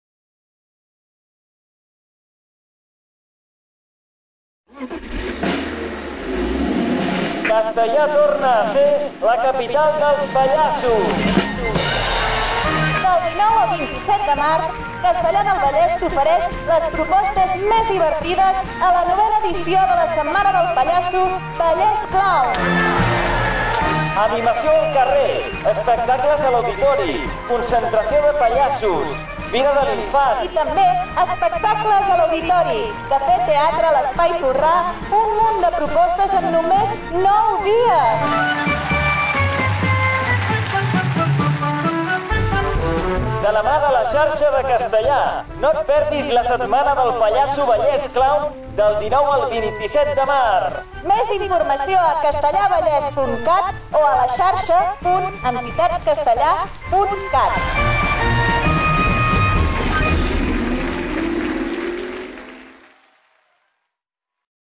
Setmana del Pallasso 2011
Del 19 al 27 de març de 2011, participa de la 9a edició de la Setmana del Pallasso - VallèsClown, que organitza La Xarxa de Castellar